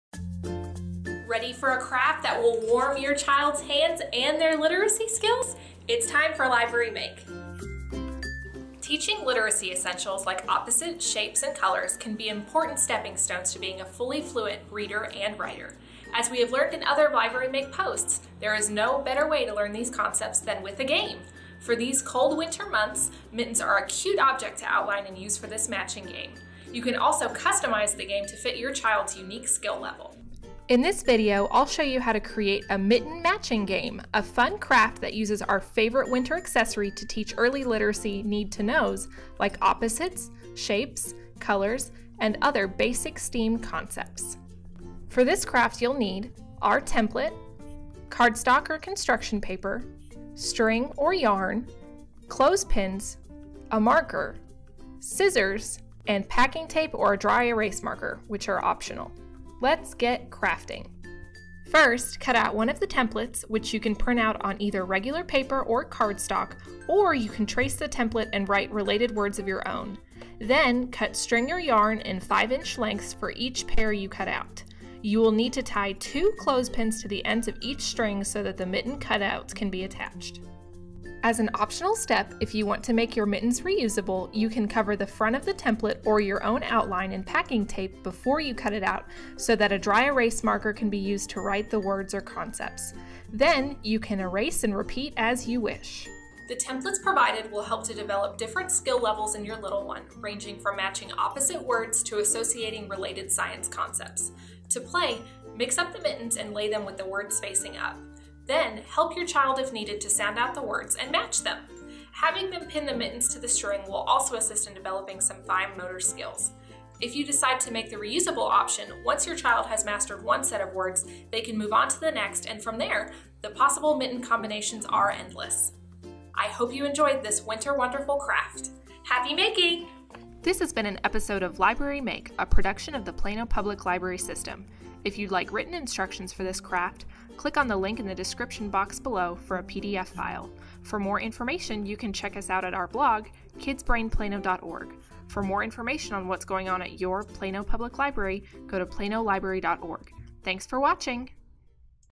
DIY Literacy: Mitten Matching Game (LIBRARY MAKE)
Make our fun matching game with your child to practice creating pairs with opposites, colors, shapes and more!  Find out how in this quick tutorial with Annie from Schimelpfenig Library.

Library Make is a video guide series for creating the fun literacy activities used at the library at home.